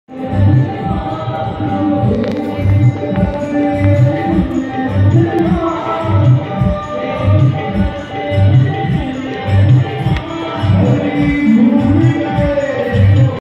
GANGA AARTI
BEAUTIFUL FLOATING DIYAS IN GANGA AARTI